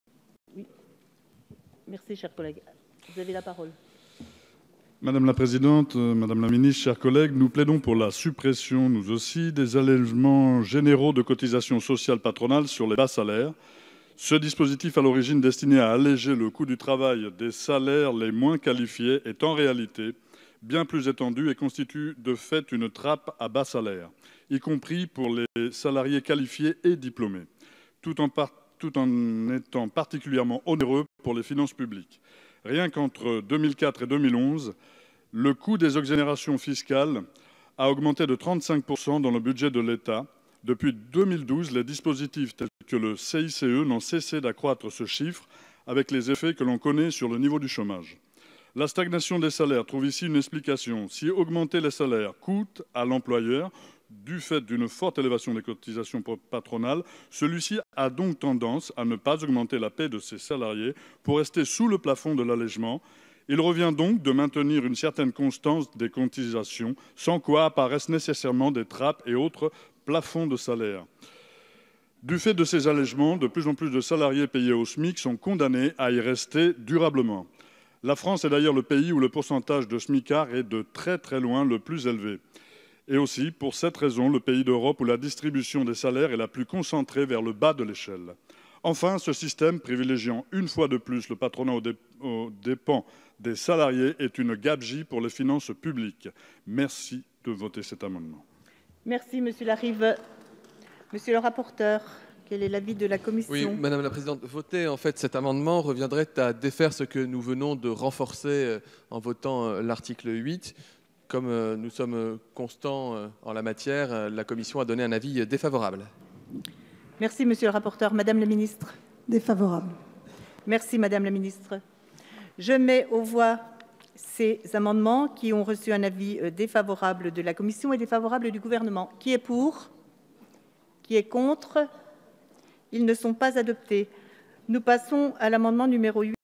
FINANCEMENT DE LA SÉCURITÉ SOCIALE 2018
Dans le cadre du projet de loi - Financement de la Sécurité Sociale pour 2018 - Michel Larive propose un amendement de suppression des allègements généraux de cotisations sociales patronales sur les bas salaires.